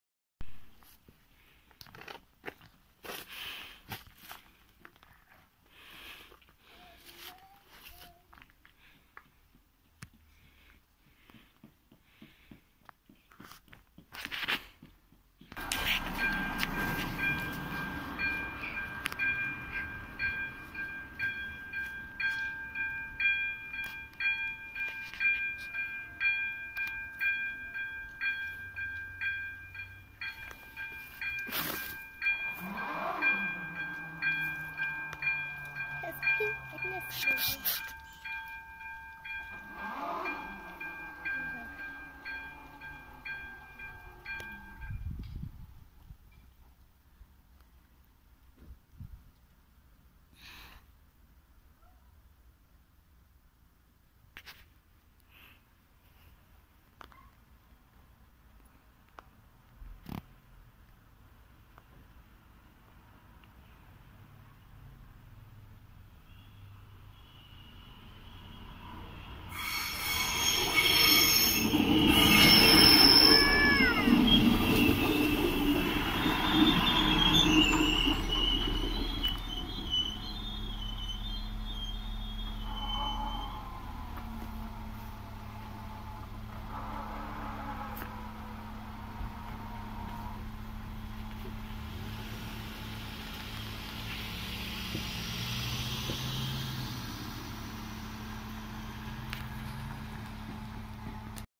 Železniční přejezd Velké Karlovice
Železniční přejezd Velké Karlovice zastávka
Výstražník:AŽD 97
Vlak : od 13283 Vsetín  Velké Karlovice
Čas: 13:39